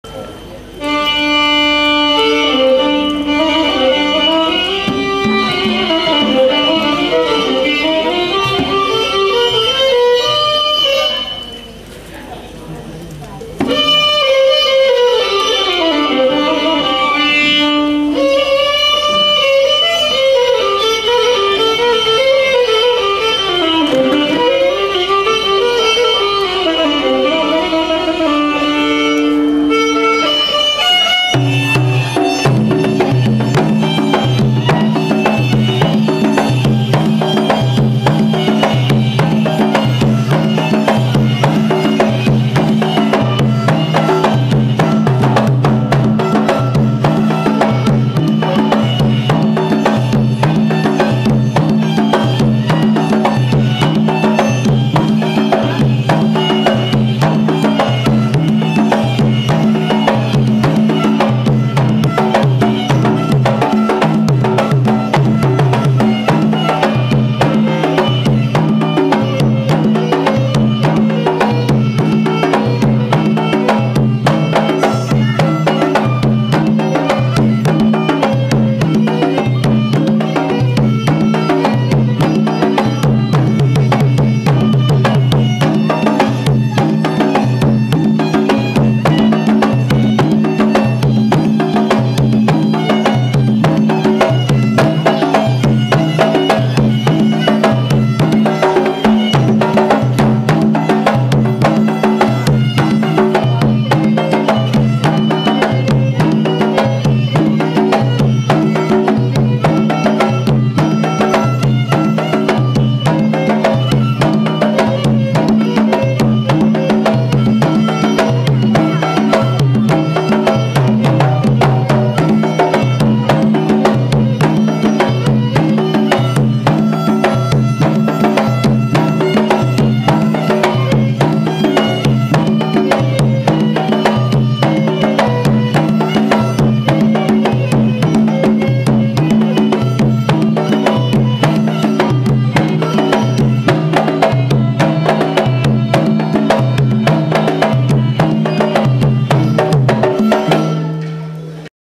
gendang biola2
permainan gendang biola di pusat latihan gayong,gurun